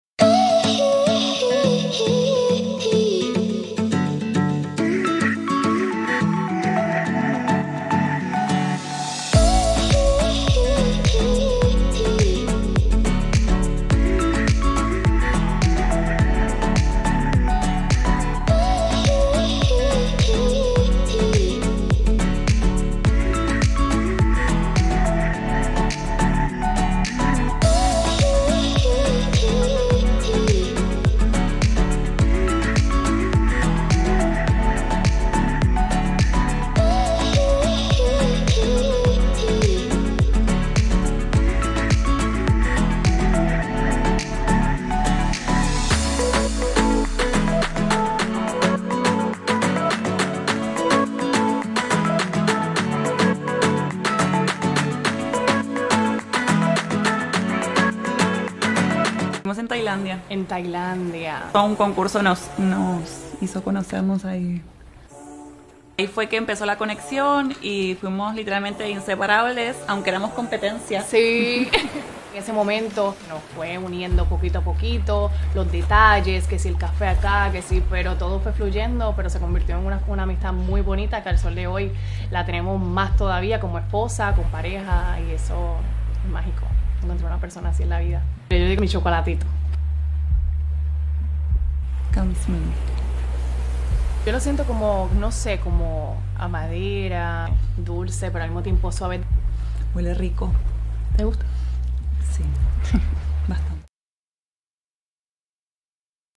La historia de amor de Miss Argentina y Miss Puerto Rico conquista a Rihanna ¡Míralas!
Mariana Varela y Fabiola Valentín llevan su amor a otro nivel gracias a Rihanna.
En octubre de 2022, las representantes de Miss Argentina y Miss Puerto Rico en Miss Grand 2020 revelaron que tenían un romance. Además, mostraron imágenes de su romántica boda.